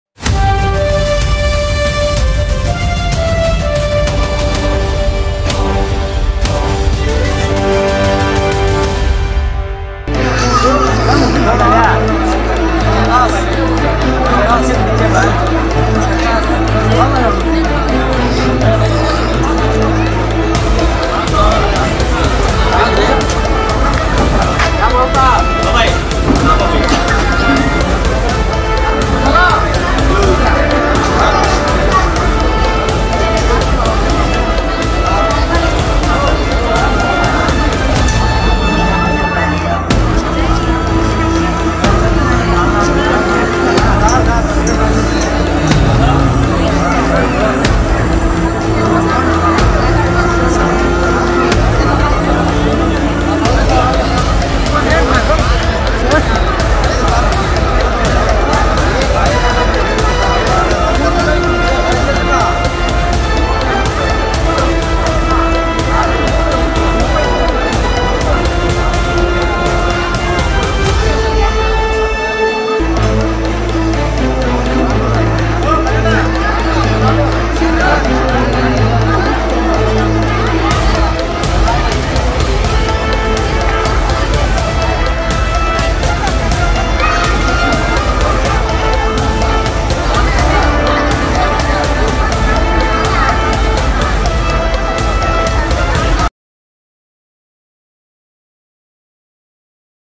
ప్రశాంతంగా ఓటింగ్ జరిగిన పెద్ద పంజాని
ప్రశాంతంగా  ఓటింగ్ జరిగిన  పెద్ద పంజాని, ప్రజలు తమ ఓటు హక్కును ఆసక్తి కరంగా వినియోగించుకున్నారు.